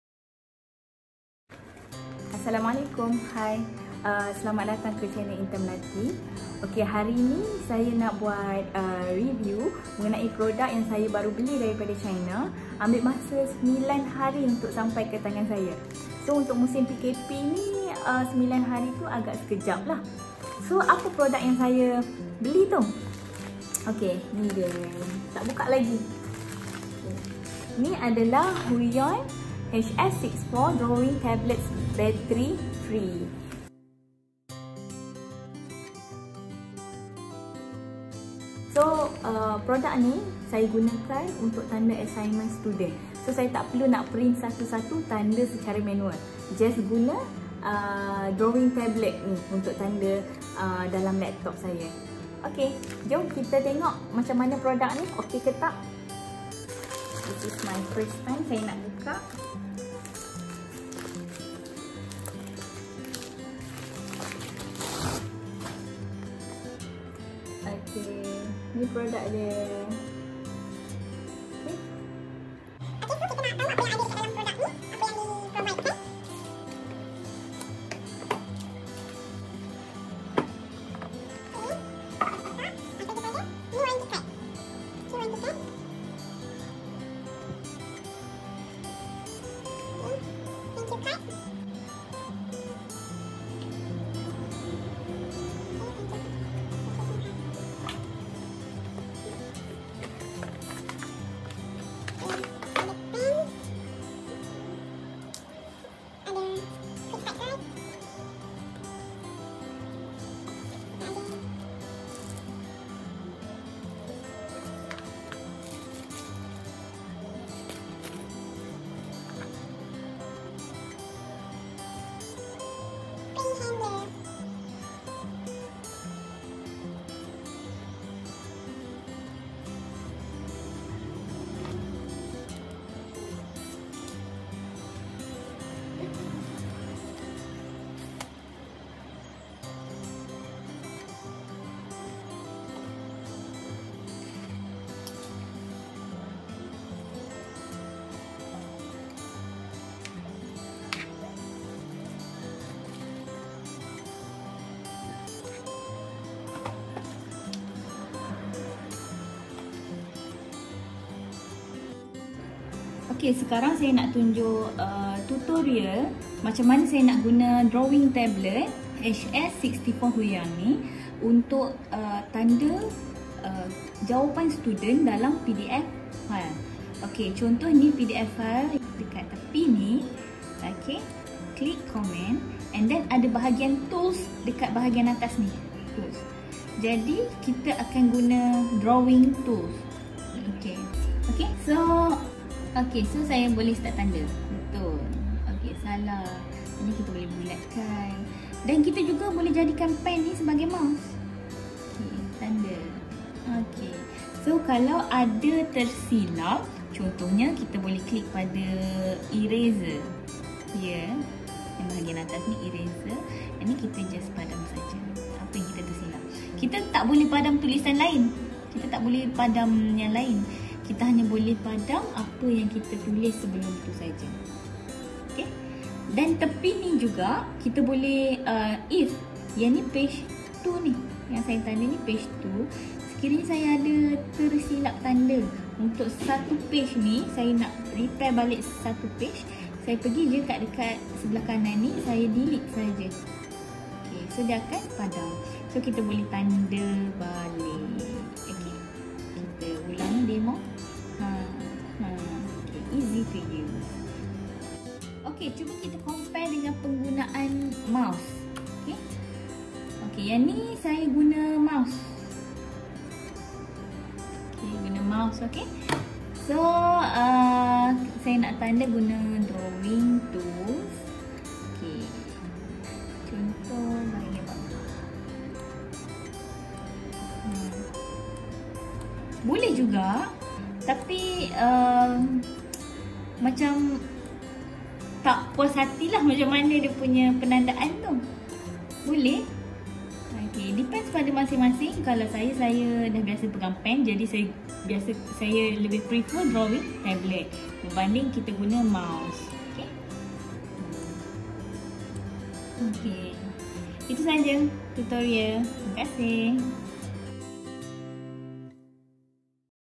Huion HS64 Drawing Tablet Review | Unboxing & Demo on PDF File
This is an unboxing, reviewing and tutorial video on how to use HUION HS64 Creative Pen Tablet for PDF file.

Melaty Amirr